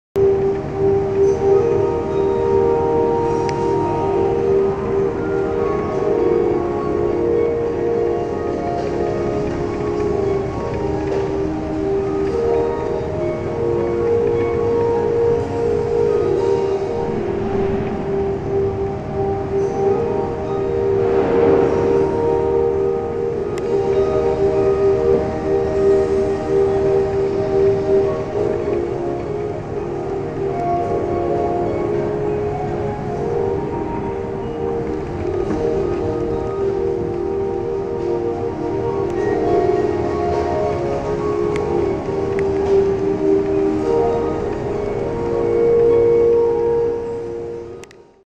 installation art video fortezza Savona
installation vidéo sur le thème de la céarmique à l'occasion d'une exposition de groupe autour du céramiste italien Giorgio laveri. Film et animation réalisés dans le studio virtuel du Moya land de second life